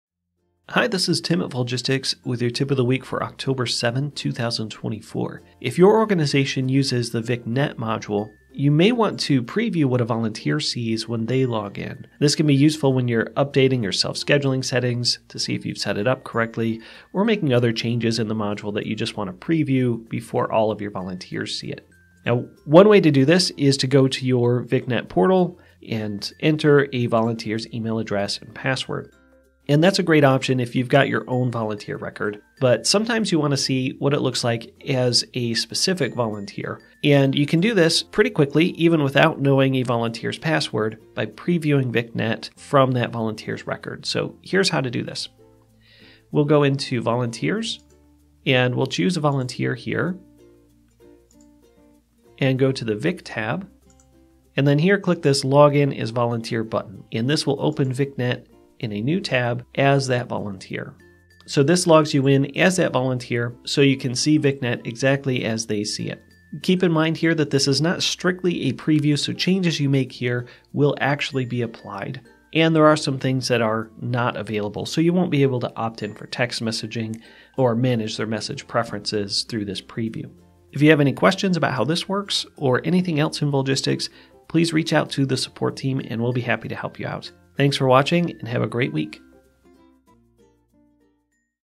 Preview What Volunteers See in VicNet
Tip of the Week October 7, 2024

If your organization uses the VicNet module you may want to preview what a volunteer sees when they login. This can be useful when you're updating your self-scheduling settings to see if you've set it up correctly.

However, you can do this even faster and without knowing their password by previewing VicNet from the Vic tab of their record. Here's how to do this:

1. Select Volunteers from the Menu.
2. Find the volunteer record.
3. Choose Vic tab.
4. If the VicNet section shows "Access is OK," click the Log in as Volunteer button.

If the tab does not show "Access is OK," VicNet Ground Rules may be helpful.